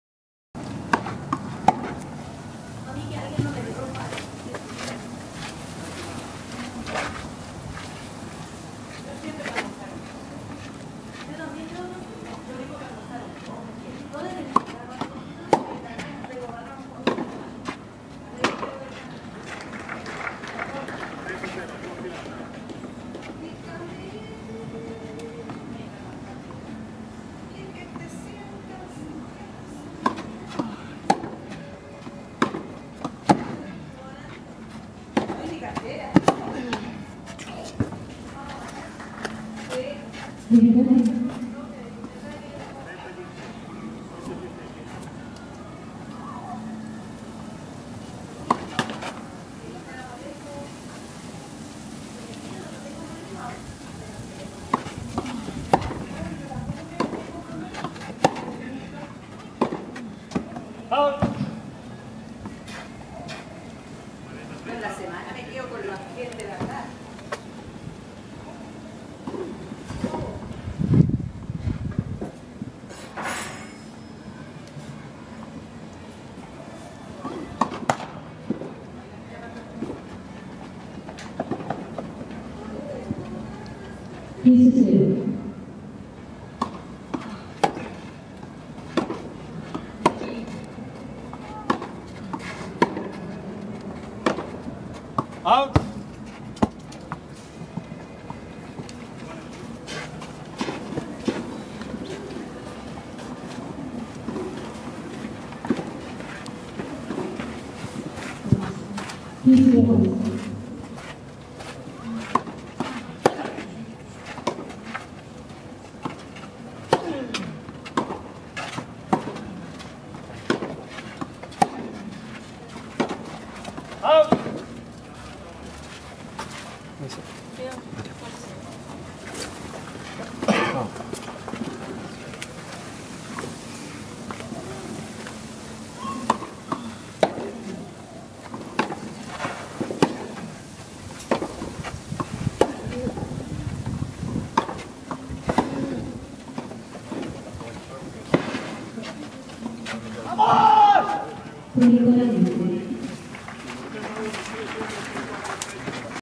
Julio Peralta-Jozef Kovalik 3º Set Último Game. Cachantun Cup 2013. Qualy 1ª Ronda
02-03-13.- Julio Peralta-Jozef Kovalik 3º Set Último Game. Cachantun Cup 2013 - Santiago (Chile). Qualy 1ª Ronda. Realizado por Miguel Gutiérrez para